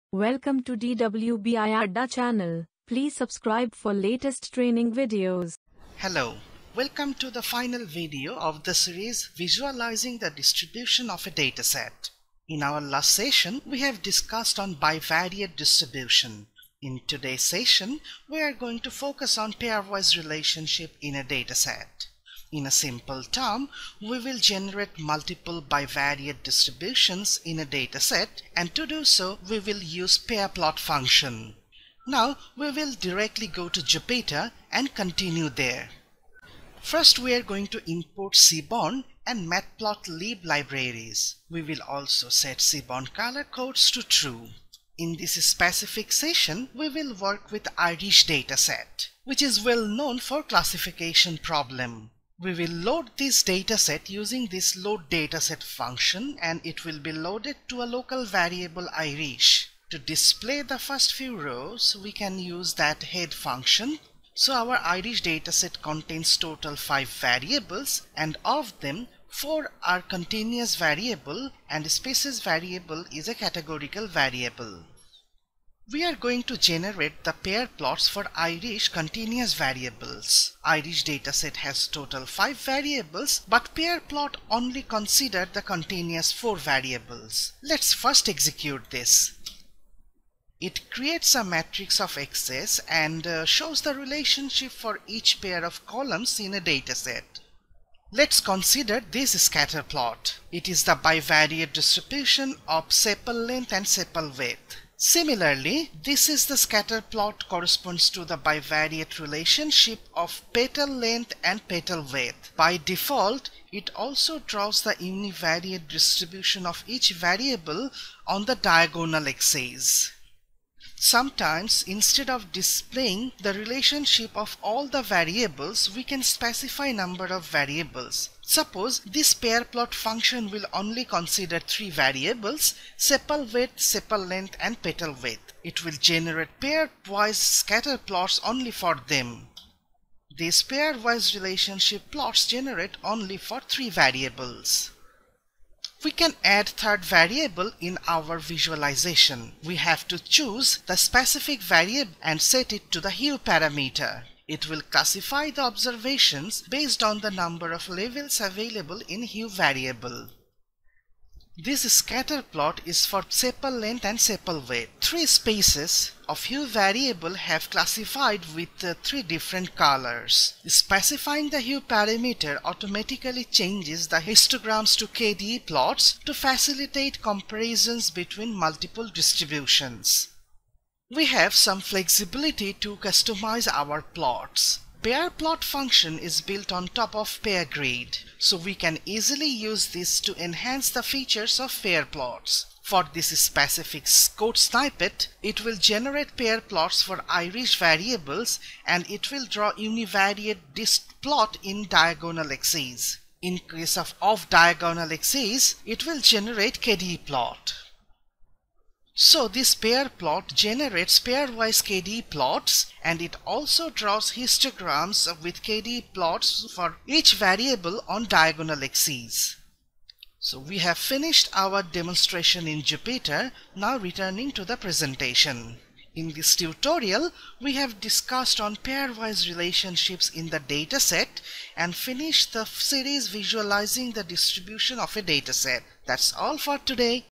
Pairwise relationships in datasets and series visualizing the distribution of a dataset
Welcome to Dwbiadda's Seaborn tutorial for beginners, As part of this lecture we will see, Pairwise relationships in datasets and series visualizing the distribution of a dataset